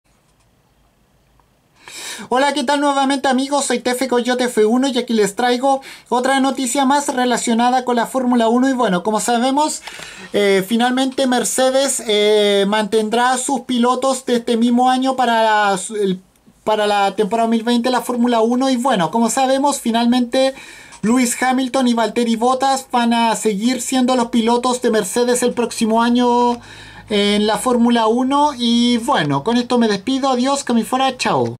Mercedes confirma a sus pilotos para 2020
Lewis Hamilton y Valtteri Bottas finalmente estarán el próximo año en Mercedes para la Fórmula 1